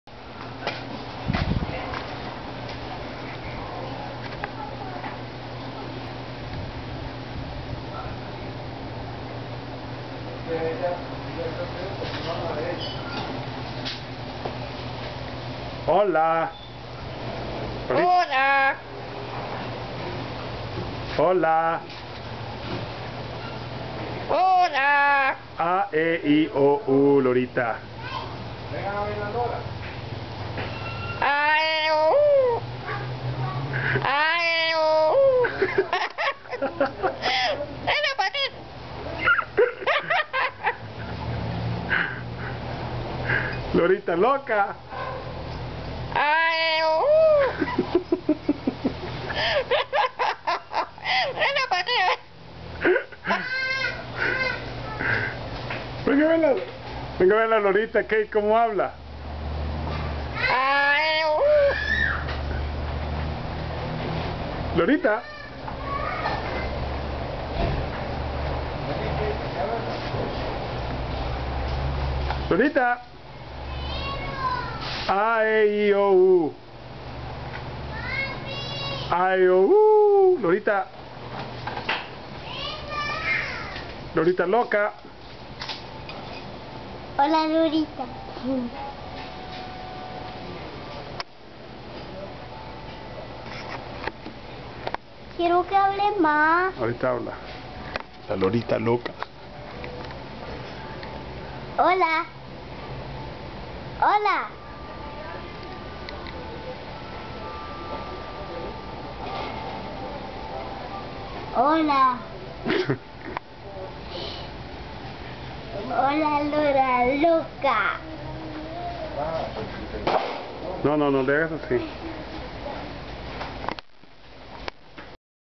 Costa Rica Liberia.. talking bird
This berd talks like a human... is funny as hell